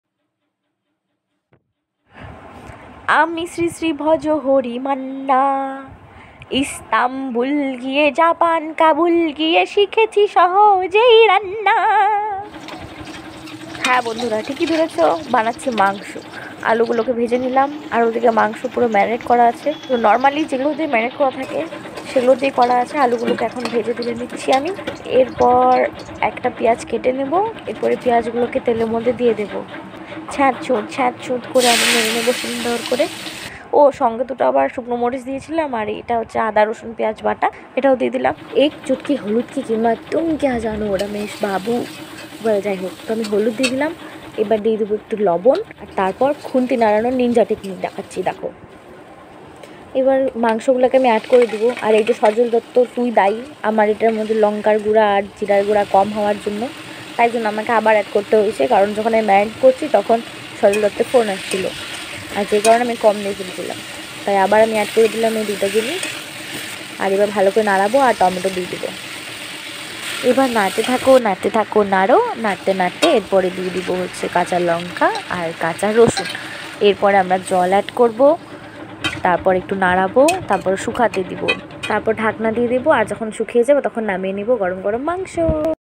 সজল দত্ত এটা কি করলি তুই ?🤔🤔😳😳
সজলের জন্য কম হলো জিরা আর লঙ্কা গুরা ।  ranna bannar funny  video banate besh mojai laglo